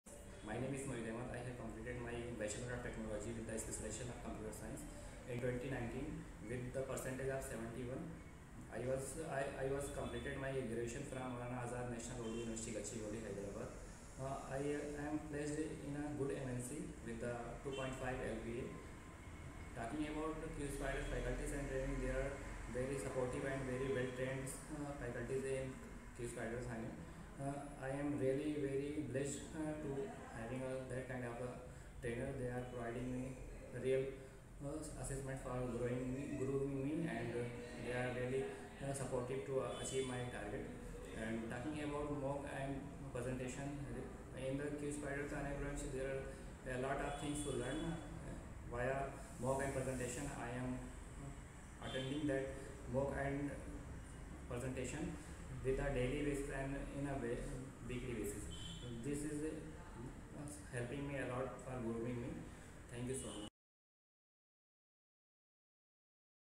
qspidersthane​​​​​​​​​​​​​​Jspidersthane MOID AHMAD (VideoTestimonial byQspidersThane
we heartily congratulate our student MOID AHMAD for getting placed from QSpiders Thane even during this COVID pandemic.🎉

Here is what he has to say about his  experience in Qspiders/Jspiders Thane about the teaching, faculty and the placements.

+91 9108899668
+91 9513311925